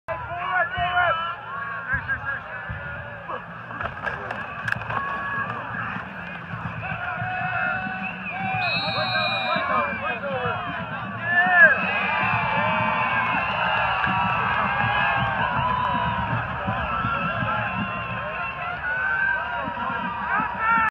Football: Columbine barrels in for short TD by Adam Harrington
Columbine junior Adam Harrington scores on 3-yard TD run late in the 2nd quarter against Ralston Valley. The Rebels took a 22-0 lead at halftime.